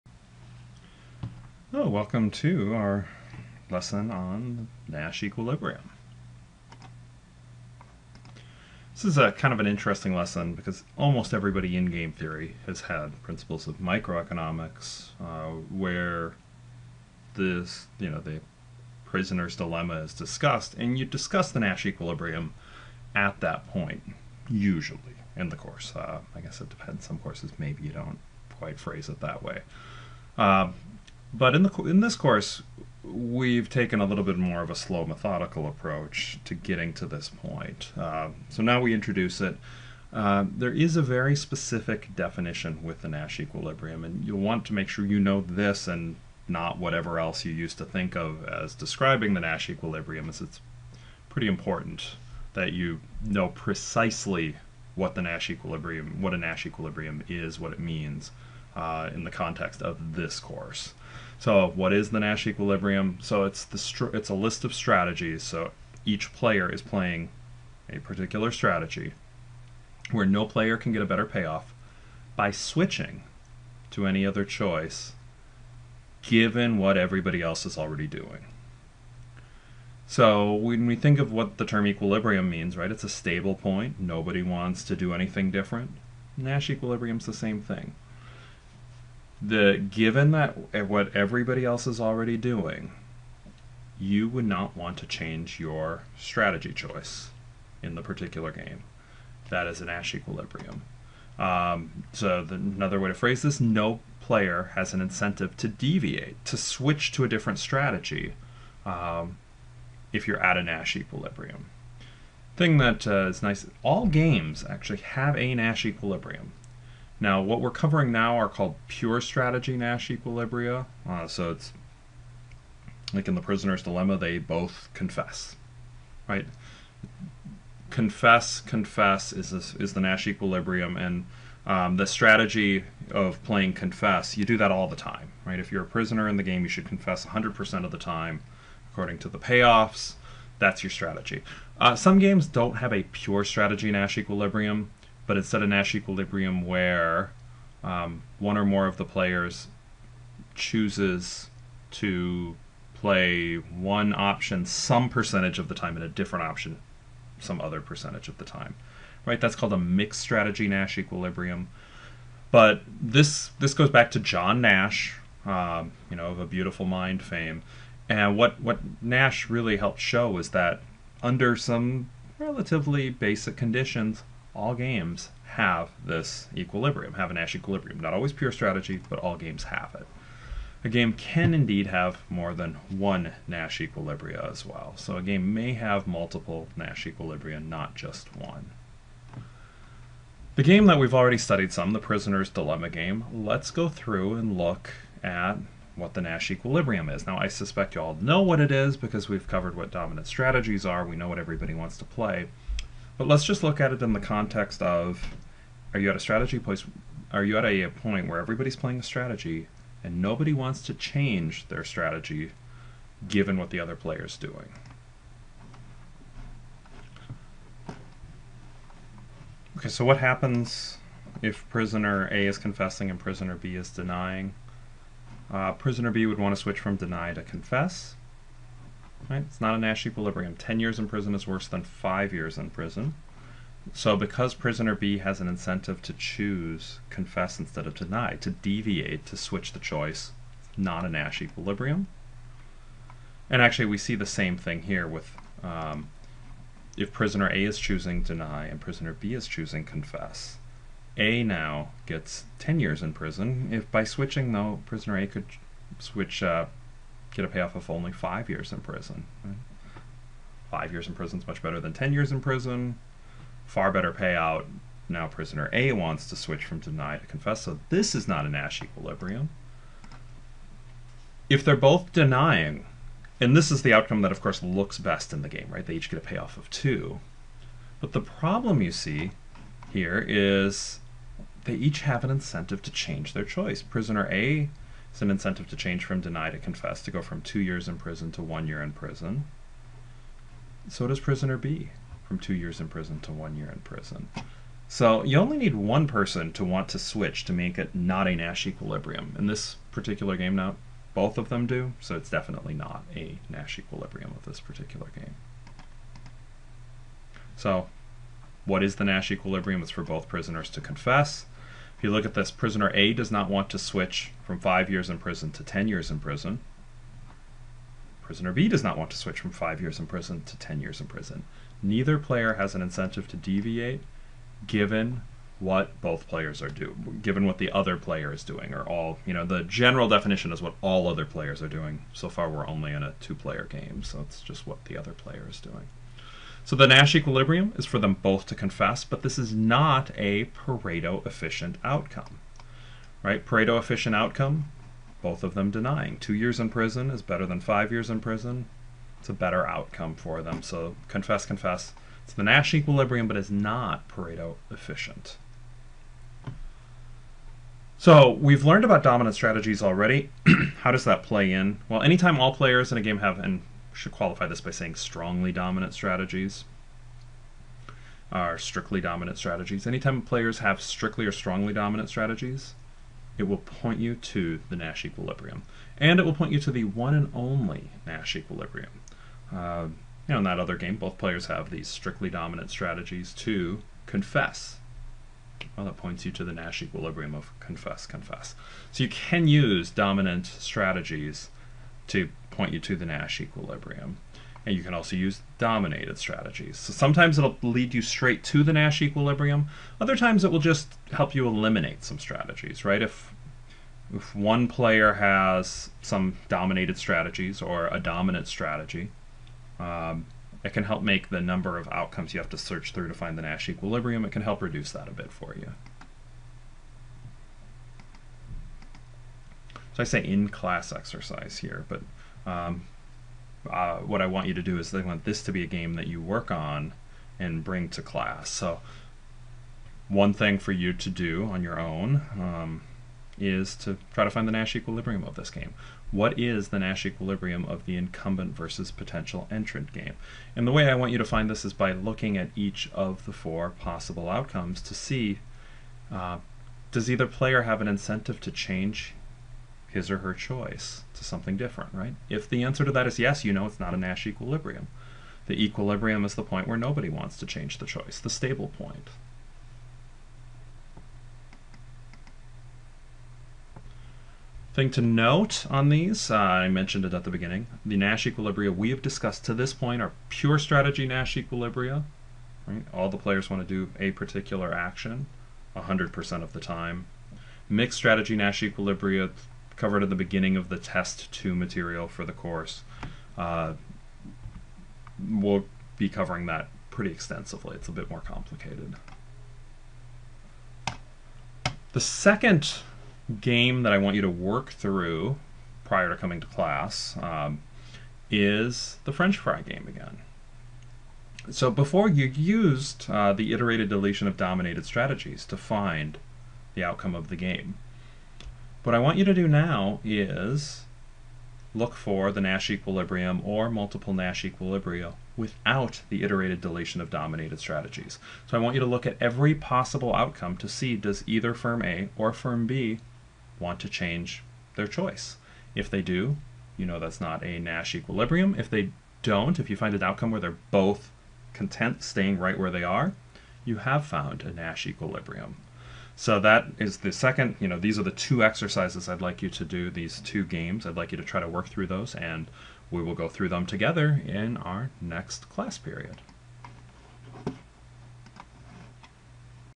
Game Theory - Nash Equilibrium
Dr. Rousu teaches undergraduate Game Theory. In this lesson, he covers Nash Equilibriums with examples.